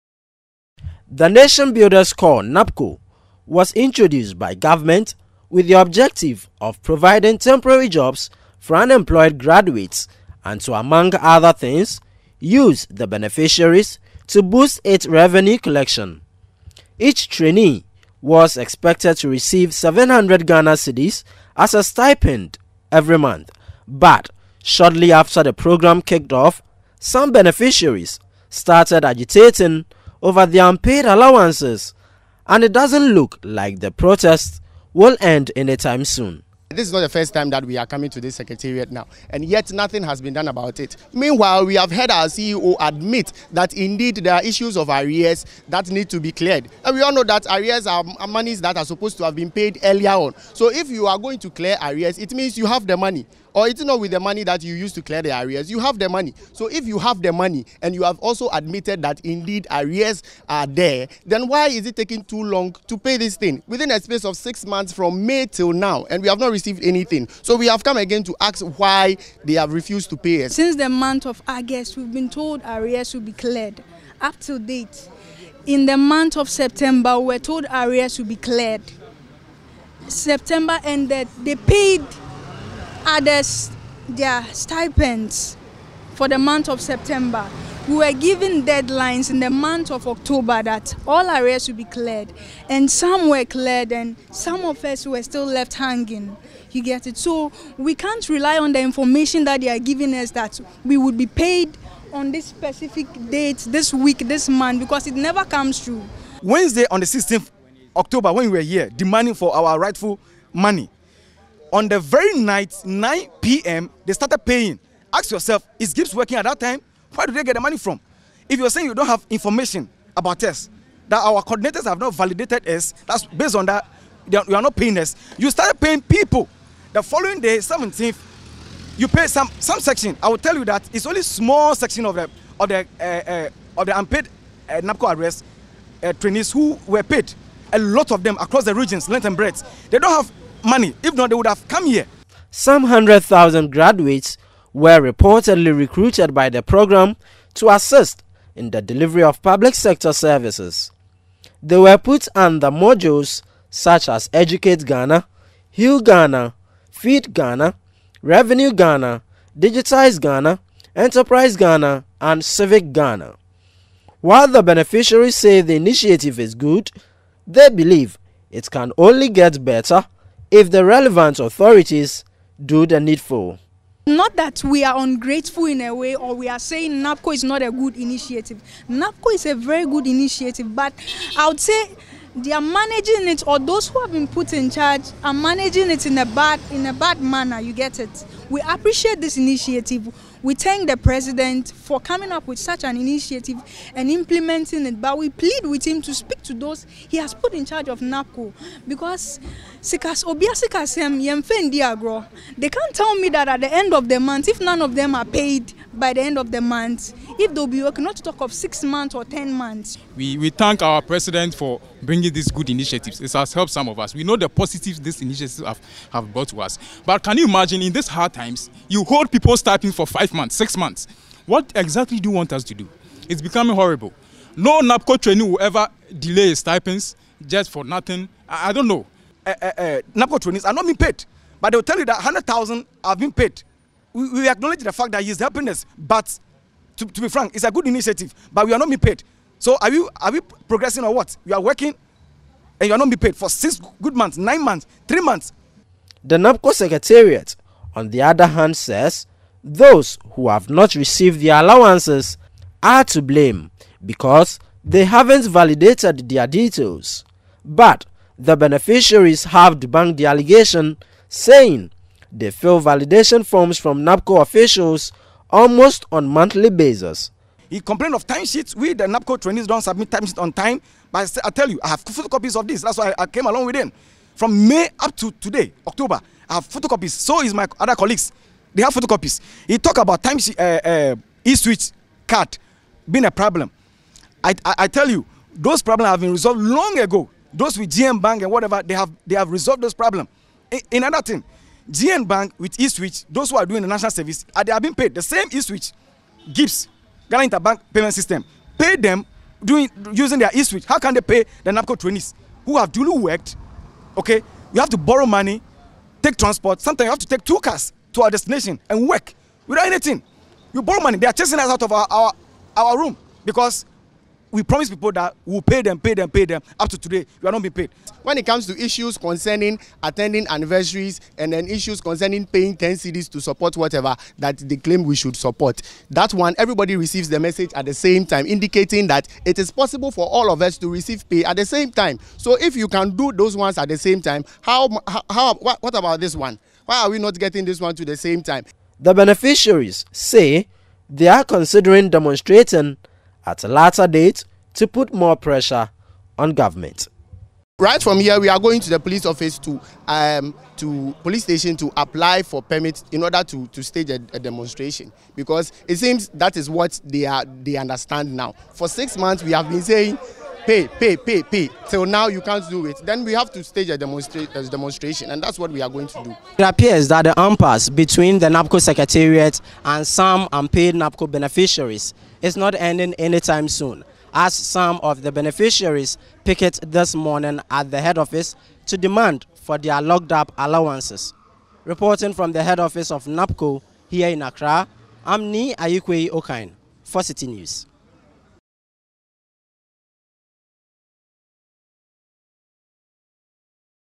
NABCo beneficiaries picket at head office over delayed allowances | Citi Newsroom
Some unpaid Nation Builders Corps (NABCO) trainees on Monday picketed at the NABCO head office over their delayed allowances.

The trainees earlier petitioned their employer to release the allowances which had not been paid since November 2018.